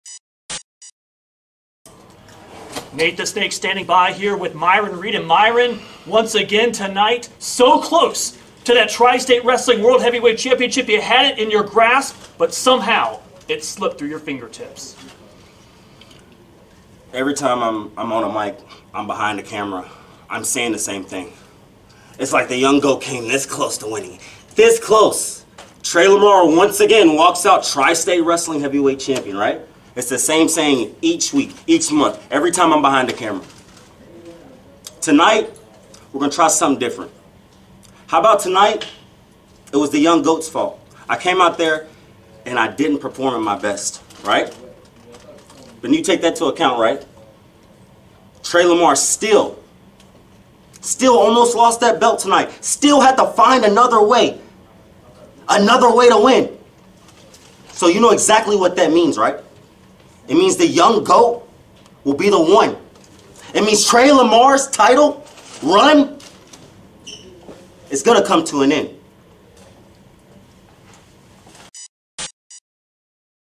Myron Reed Responds to Tre' Lamar Retaining the TSW Heavyweight Championship on April 27
The Young G.O.A.T. Myron Reed responds to Nate "The Snake" Pelley's questions after a controversial--yet successful--title defense by Tri-State Wrestling Heavyweight Champion Tre' Lamar.

Myron Reed accepts responsibility, stating he didn't perform at his very best level. Taking that into account, Myron was still so close to a victory. He's certain that Tre' Lamar's days as Tri-State Wrestling Heavyweight Champion are numbered. And he's certain it'll be Myron Reed who defeats him.